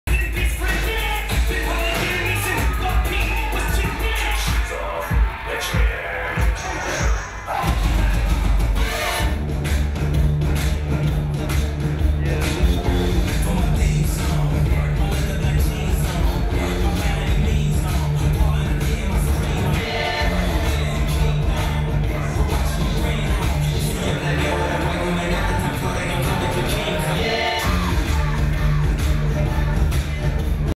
Models walk the runway at Betsey Johnson - Spring 2014 Me...
Models walk the runway at Betsey Johnson - Spring 2014 Mercedes-Benz Fashion Week in New York, NY, on 9/11/13.